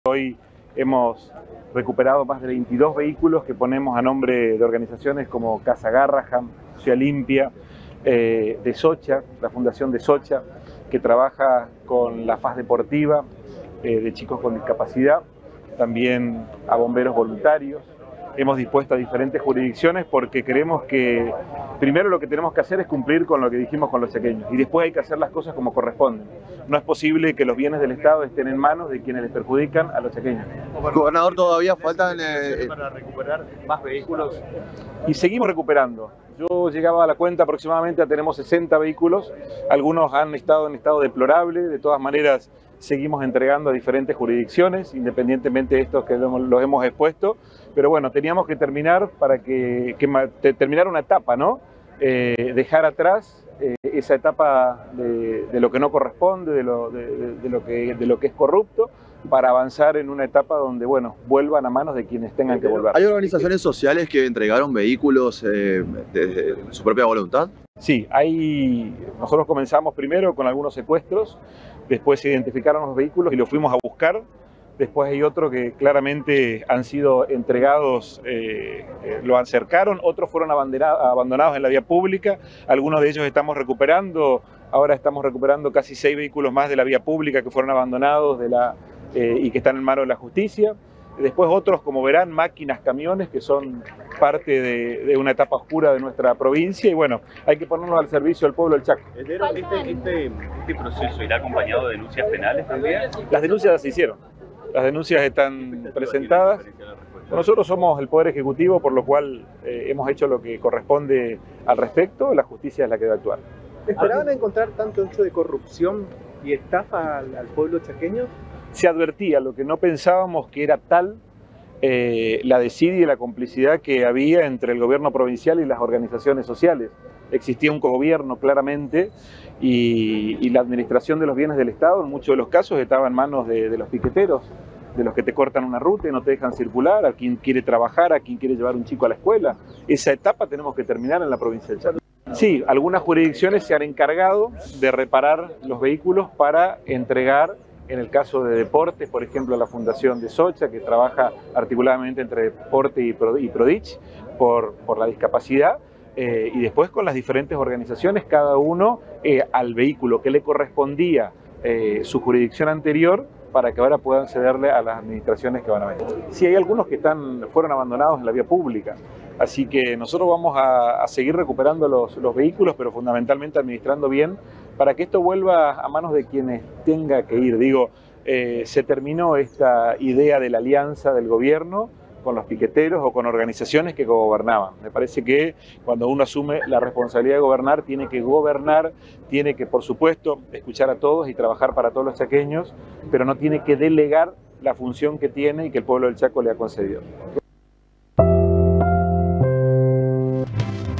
NGFEDERAL - ENTREGARON MÁS DE 60 VEHÍCULOS RECUPERADOS
En conferencia de prensa realizada en la Plaza 25 de Mayo de la ciudad de Resistencia, el jefe del Ejecutivo provincial precisó que en lo que va de la gestión recuperaron más de 60 vehículos oficiales.
NOTA: LEANDRO ZDERO - GOBERNADOR DE CHACO